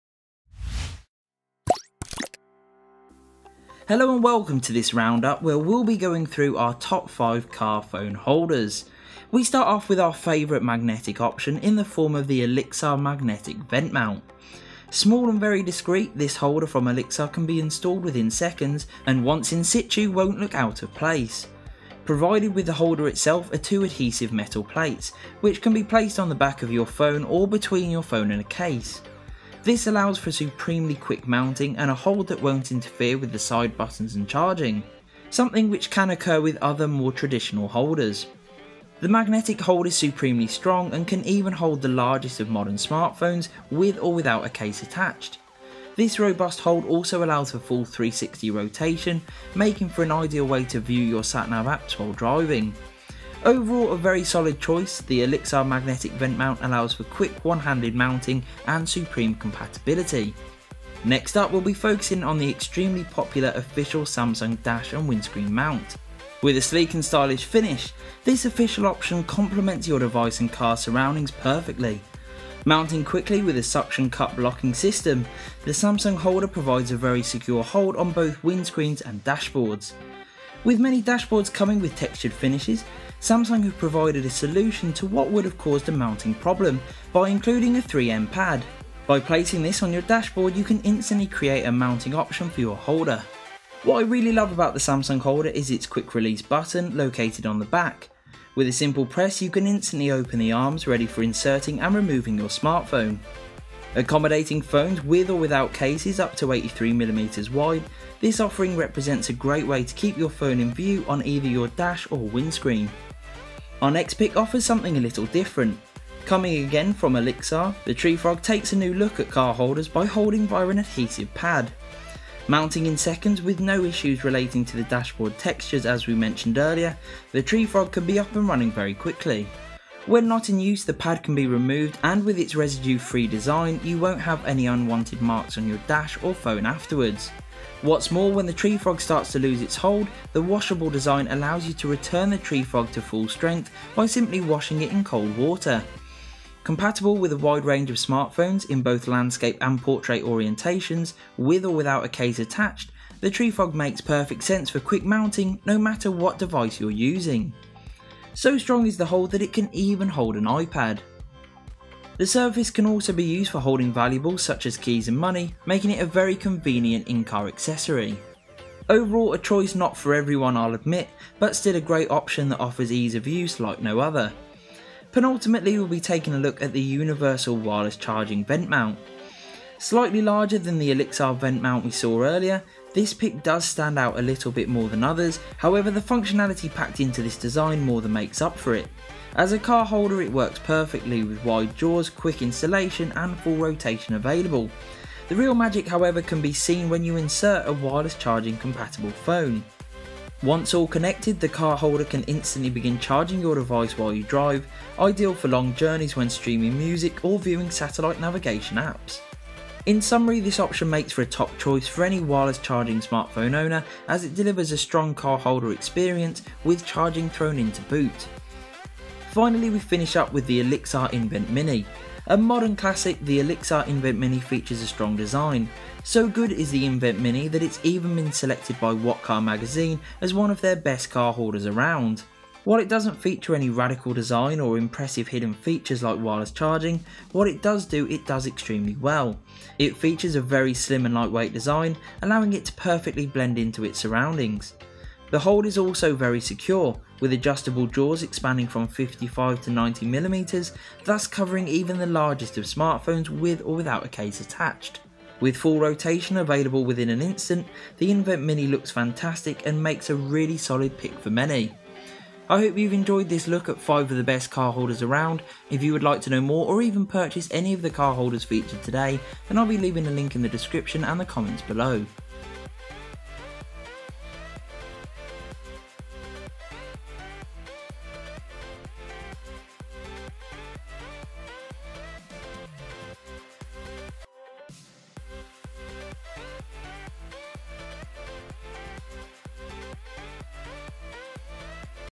Best Car Phone Holders You Can Buy
In today's video round-up we'll be looking at our top 5 car phone holders. With all the best styles and brands, including Vent, Dash, Windscreen and wireless charging vent mounts from brands such as Olixar and Samsung.

Social Links...